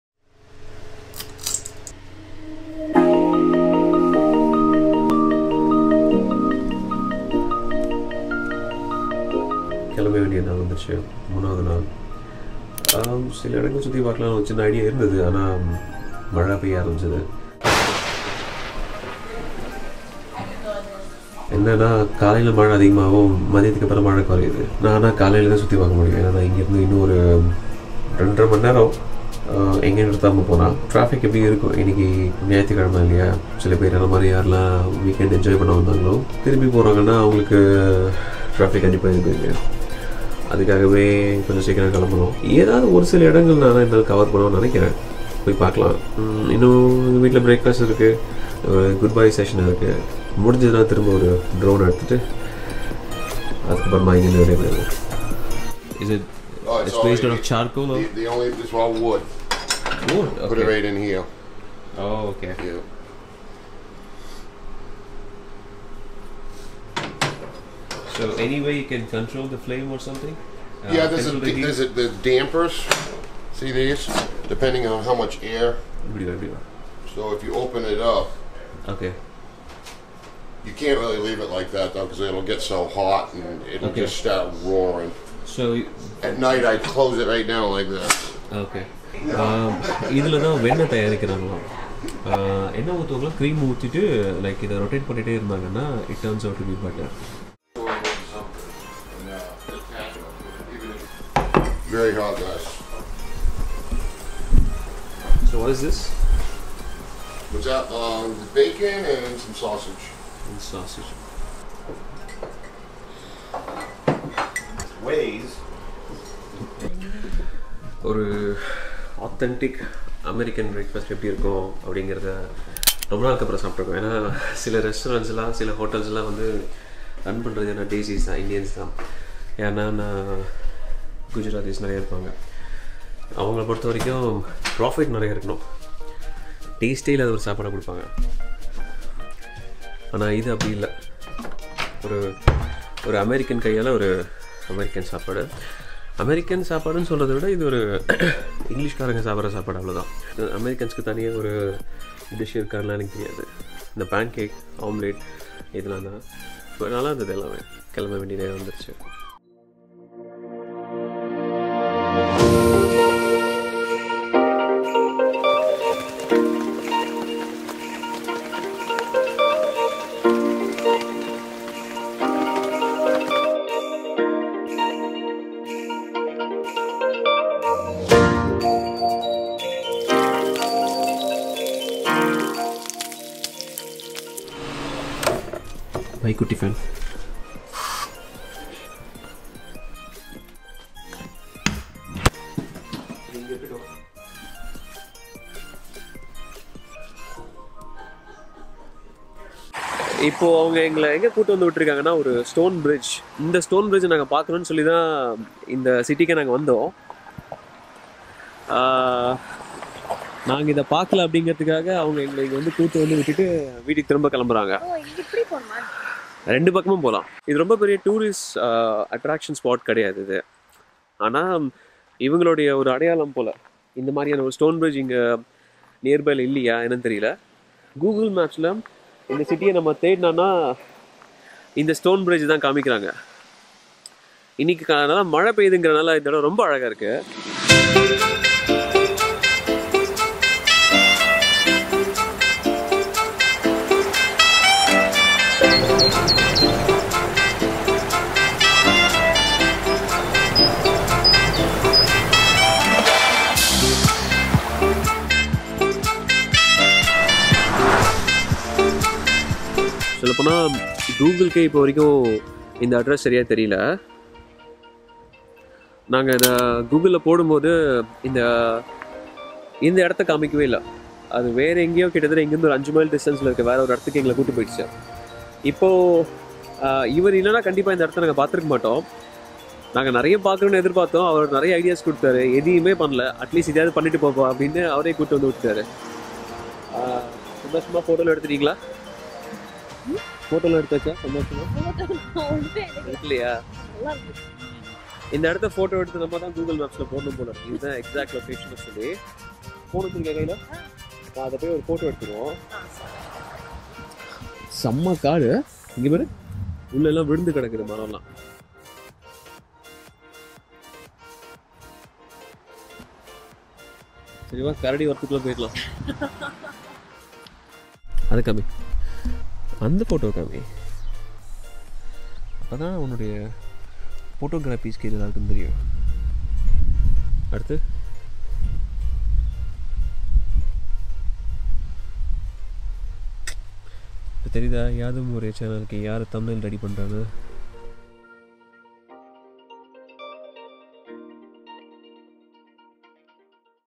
Day 3 at HummingBird Hill | Yaadhum Oore | Tamil Travel Vlog
Our last day in HummingBird Hill at New Hampshire.

Here's the list of things I use to make these videos.

My Camera
My Fav Camera
Always connected Lens
Additional Lens
Easy access Tripod